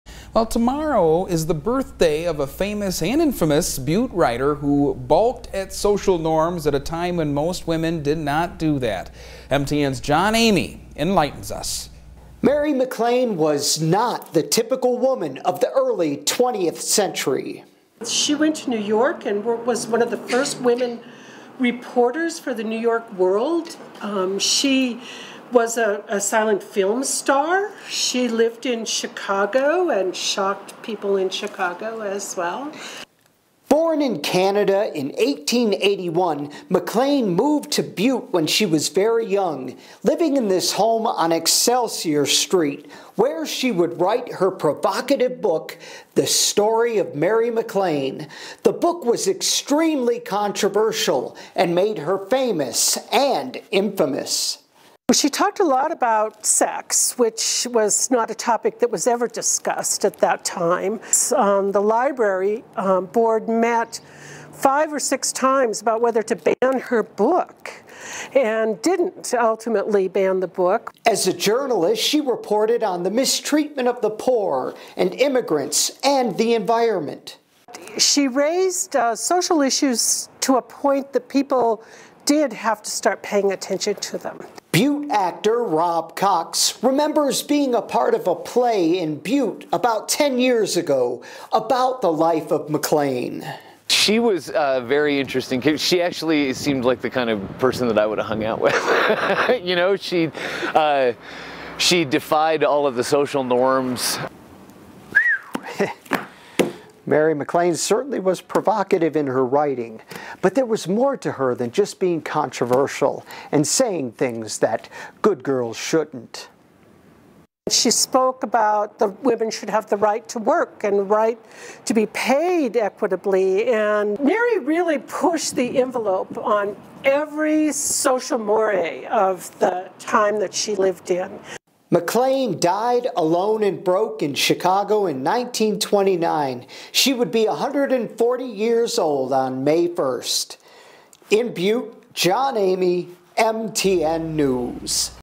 Controversial Butte writer Mary MacLane a pioneer in early women's suffrage
Mary MacLane was not a typical woman of the early 20th century.